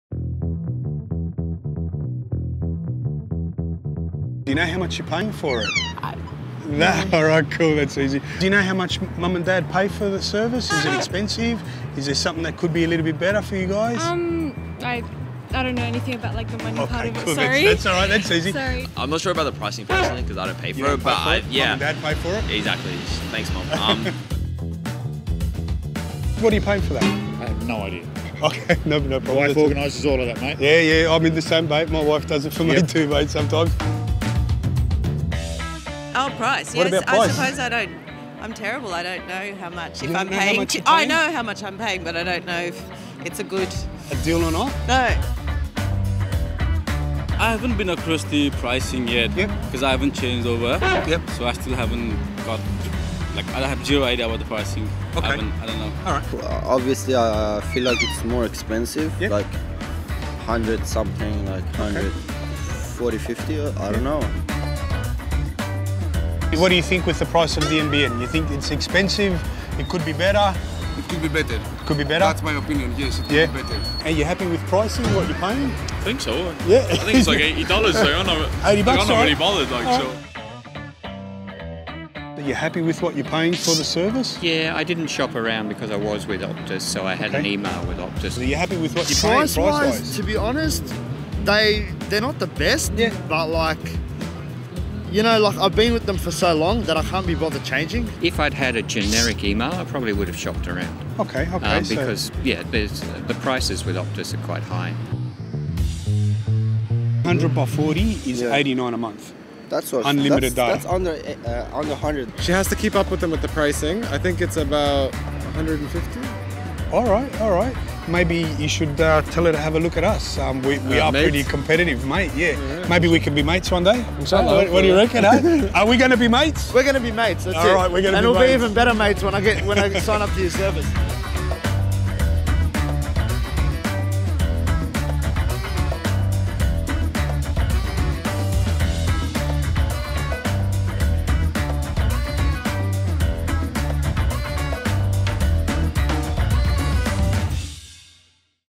Frankie hits the streets - Price
Frankie at MATE hits the streets of Western Sydney to find out what people think of their home internet connection!

What do they think of the price they pay?